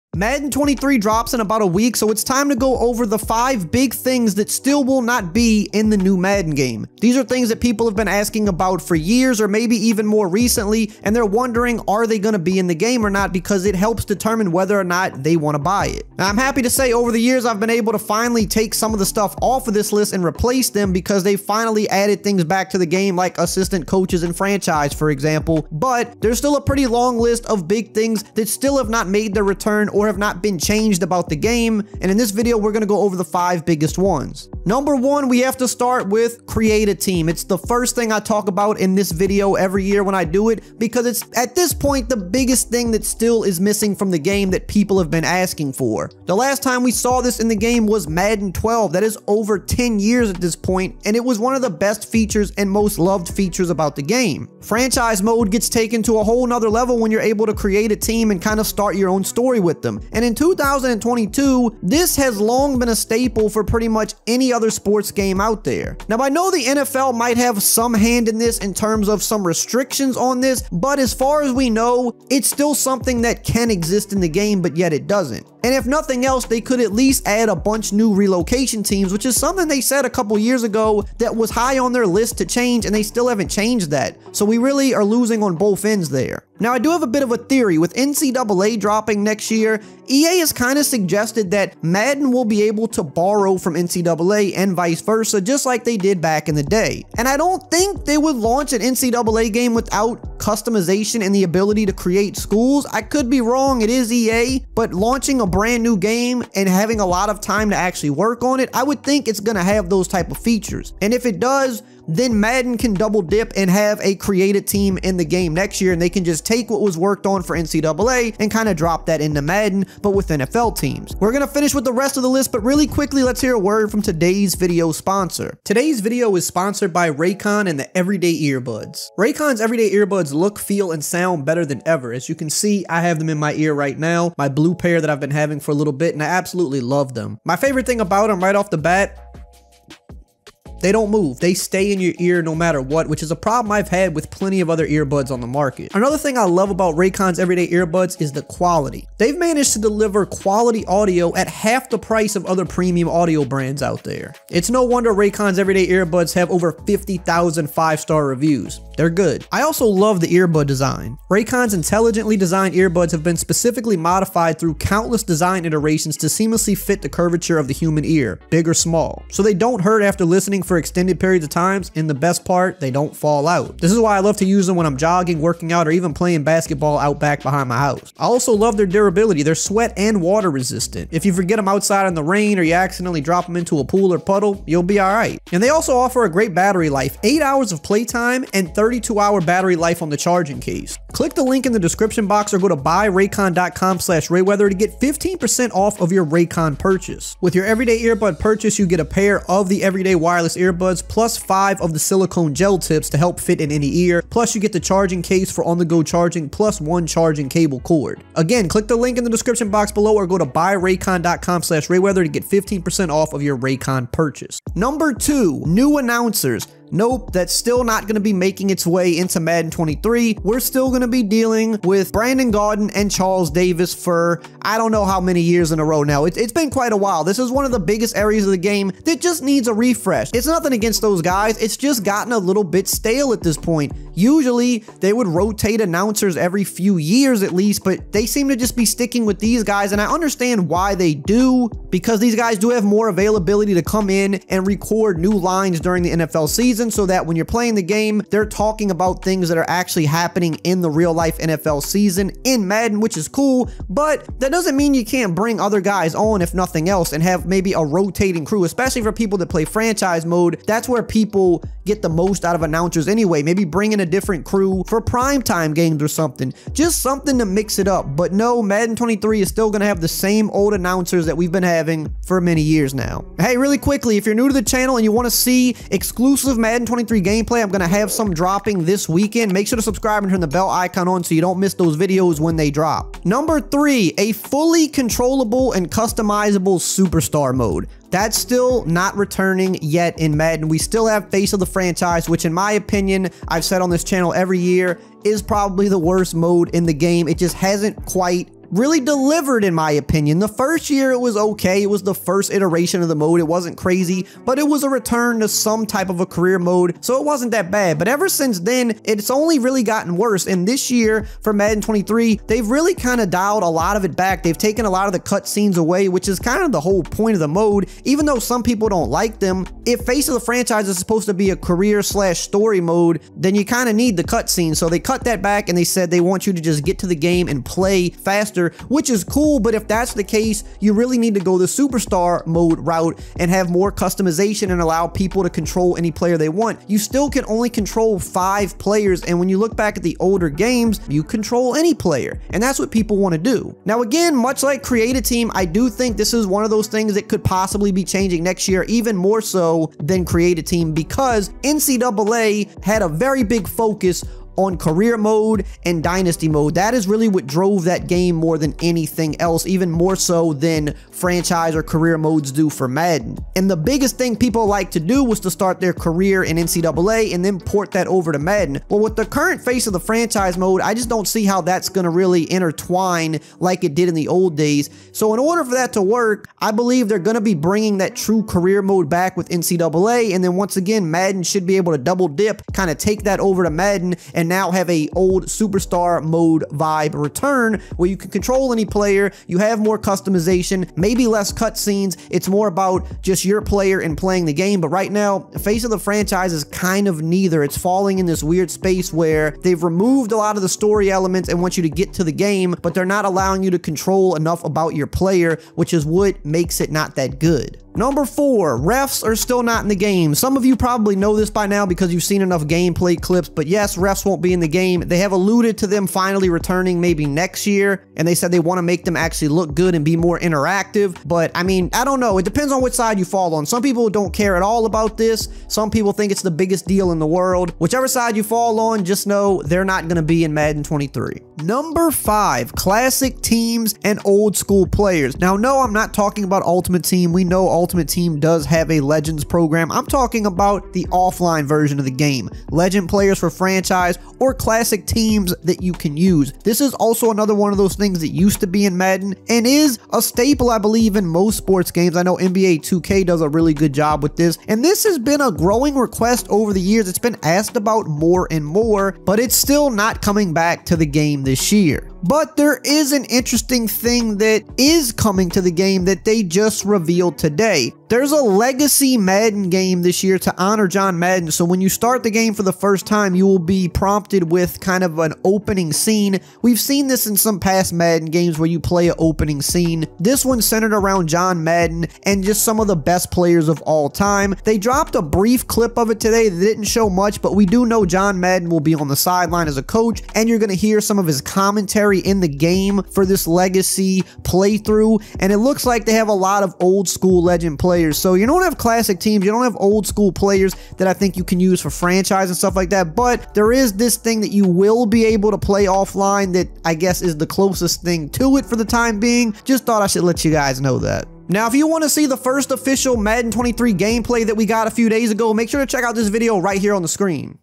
5 BIG Things That STILL Won't Be in Madden 23!
In this Madden 23 video, I'll be going over 5 big things that still won't be in the game!

MY GEAR:
Gaming Monitor